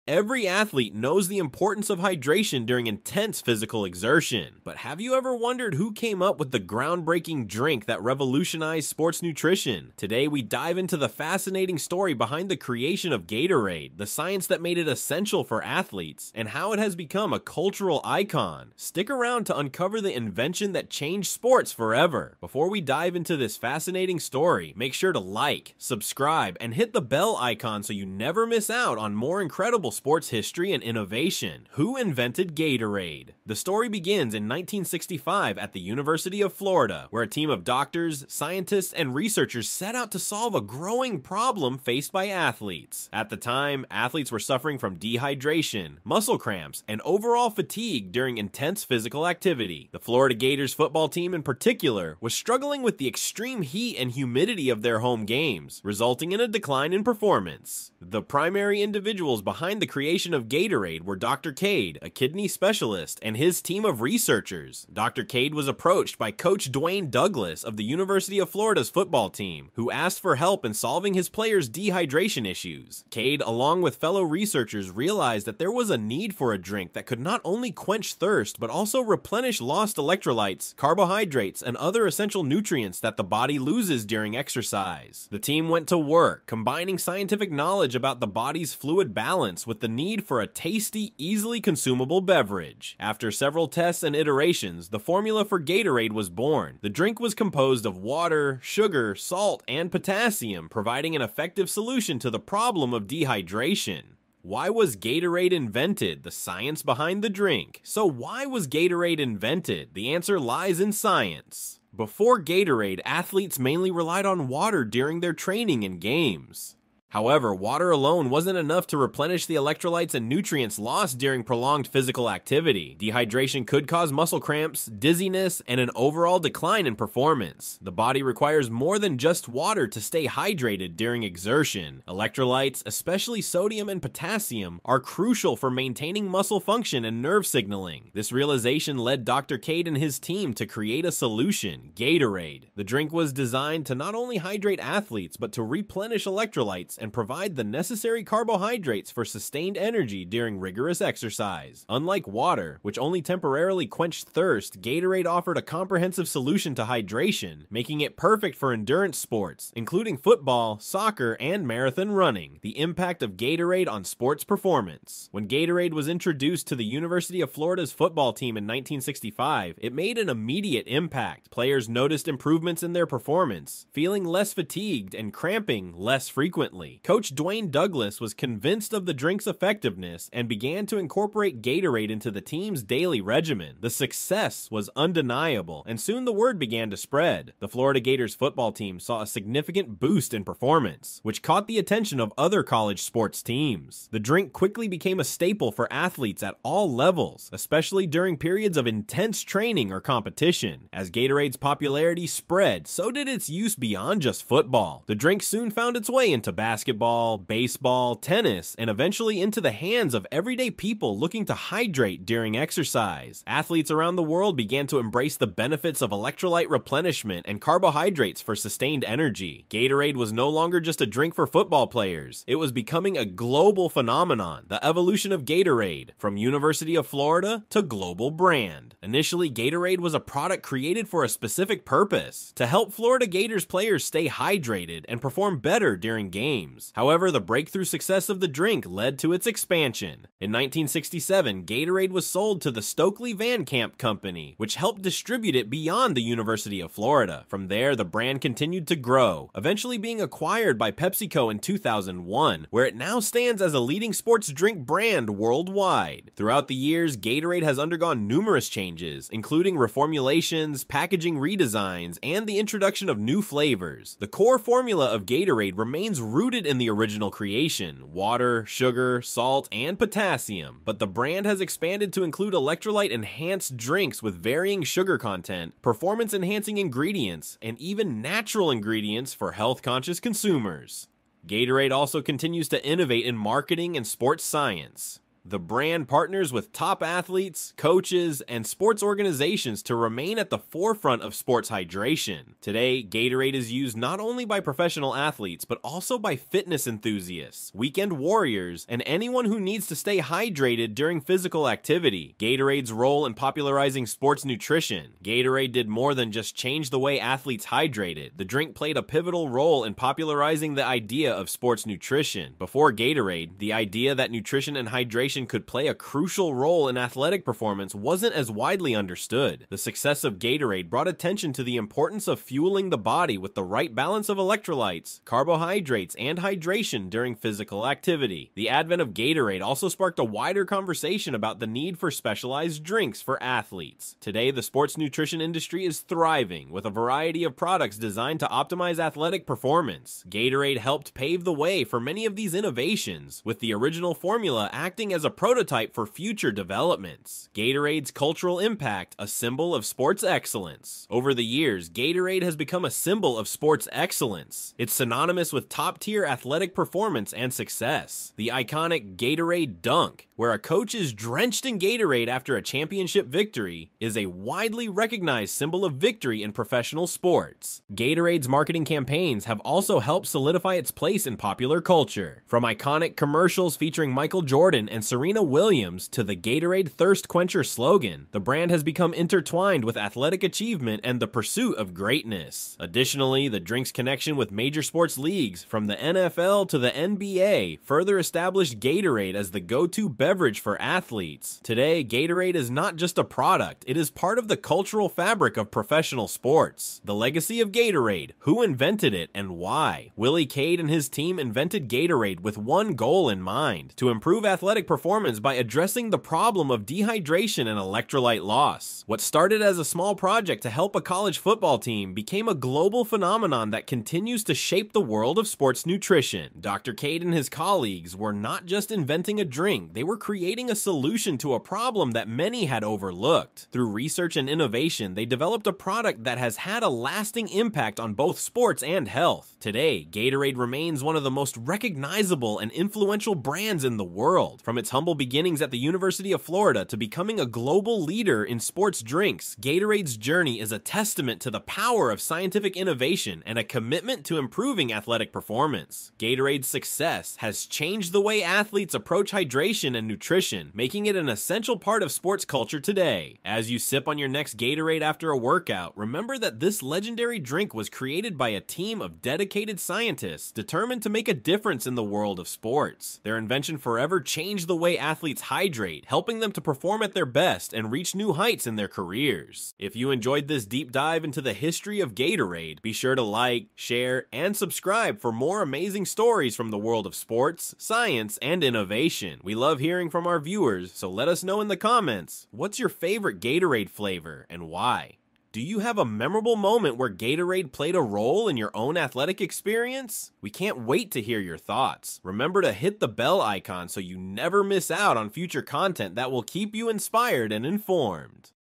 Hydration Is Key: Who Invited Gatorade? | Does it Keep You Hydrated?
In a world where sports science meets innovation, "Origins of Gatorade" takes you on a captivating journey into the creation of one of the most iconic sports drinks ever. Discover how a groundbreaking blend of scientific ingenuity and athletic need gave rise to Gatorade, forever changing the face of athletic performance and hydration.

Join us as we delve into the rich history of Gatorade, exploring its inception at the University of Florida in 1965. Uncover the inspiration behind its development: a group of scientists and doctors determined to help the Florida Gators football team battle the heat and dehydration on the playing field. Learn how their revolutionary creation became an essential tool for athletes worldwide, transcending the boundaries of sports and influencing the beverage industry forever.

In this documentary, we highlight key moments and figures in the evolution of Gatorade, including the pivotal role of Dr. Robert Cade and his dedicated team. Witness the trials and triumphs that led to the launch of a product that would become synonymous with sports hydration.

Whether you're a sports enthusiast, science buff, or curious historian, "Origins of Gatorade" offers a fascinating insight into the blend of determination, creativity, and research that fueled the creation of this legendary sports drink. Dive deep into the story of how Gatorade became more than just a beverage—it became a game-changer.